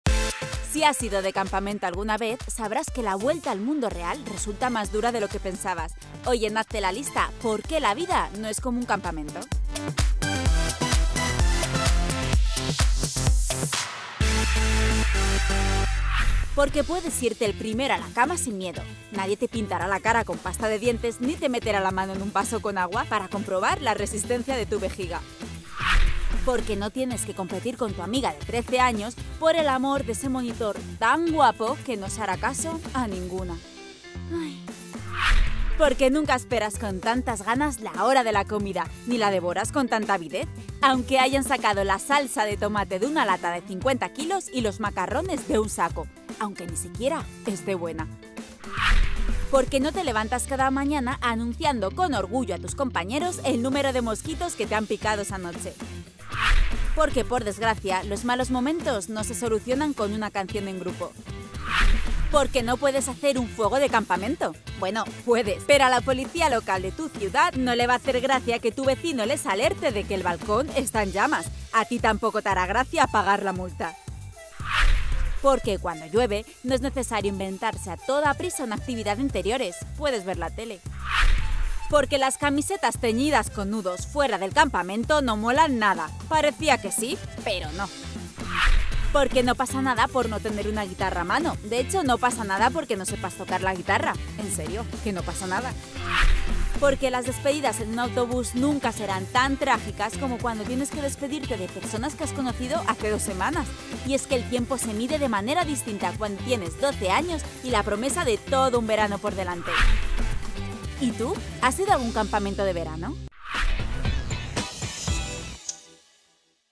10 razones por las que la vida no es como un campamento de verano
Nuevos amigos, diversión, aventuras… los campamentos de verano son una experiencia única que muchos esperábamos con ganas cuando comenzaban las vacaciones en el colegio o instituto. Pero en Hazte la lista te damos 10 razones que demuestran que la vida no es como la vivías durante el campamento y…¡menos mal!